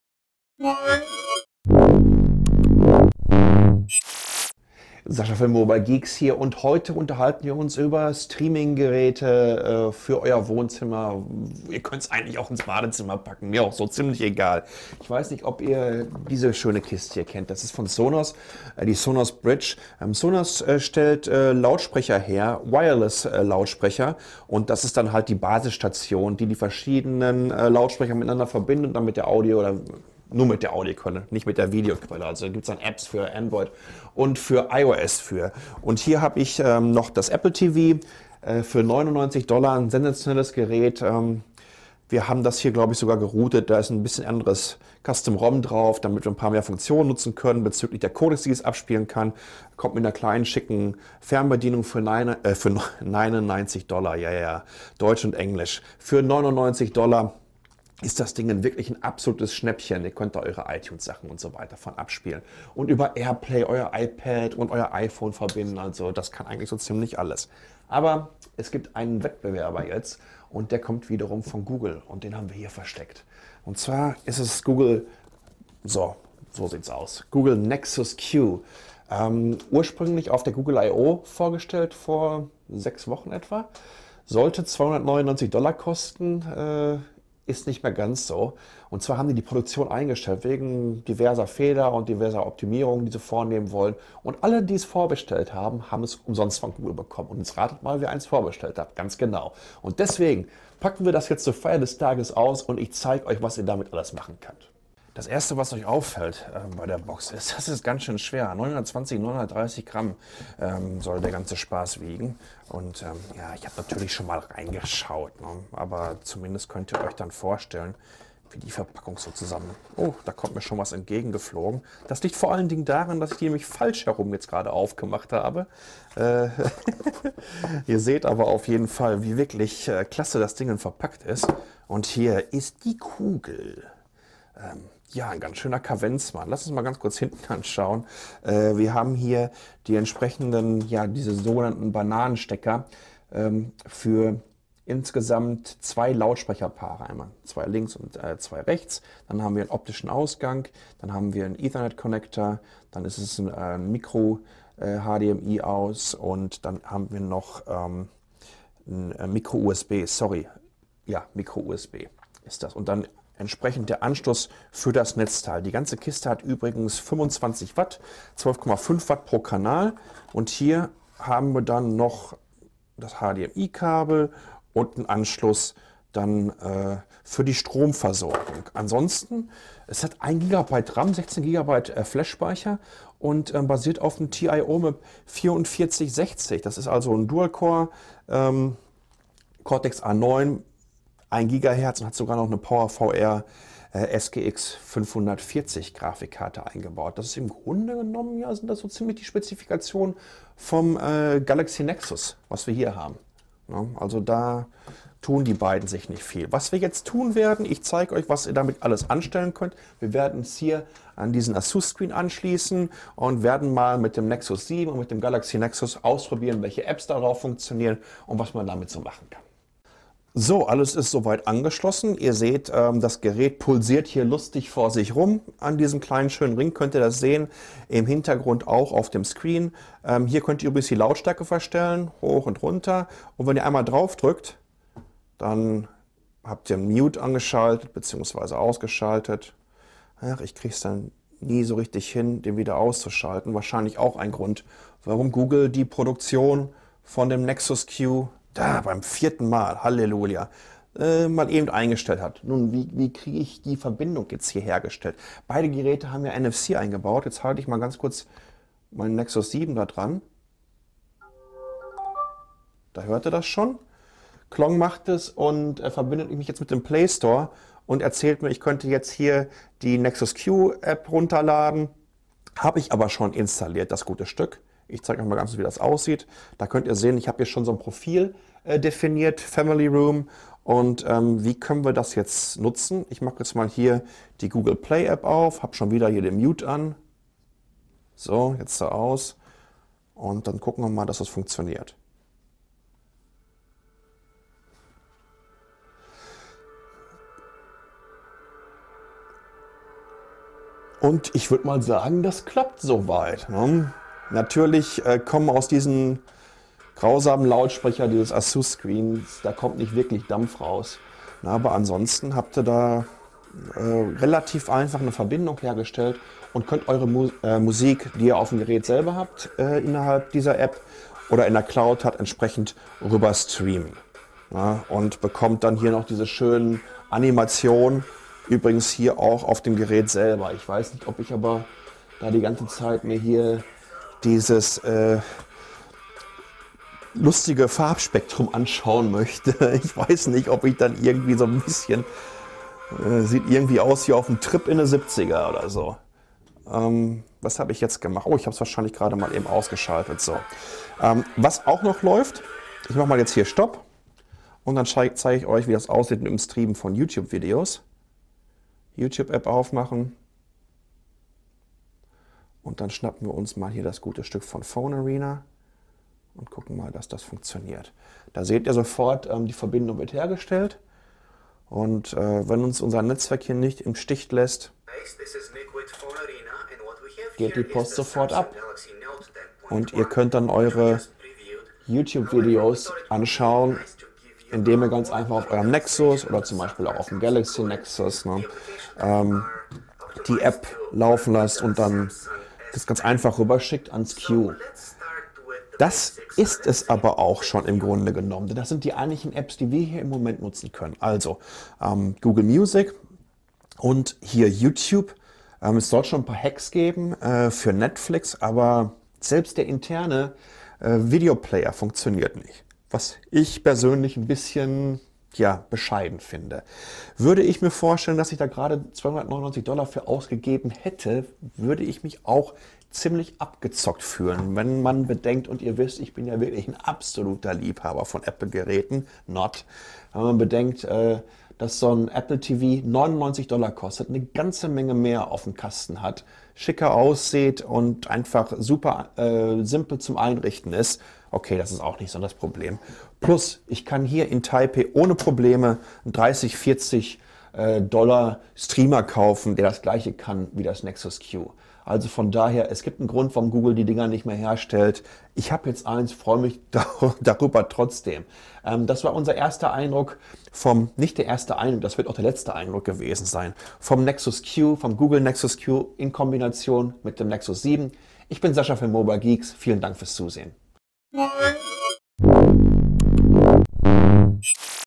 Google Nexus Q Im Test - Nexus Q Testbericht [Deutsch - German]
Nexus Q Testbericht - Wir schauen uns das Nexus Q von Google in Kombination mit dem Nexus 7 einmal genauer an und zeigen euch, was ihr damit so alles machen koennt.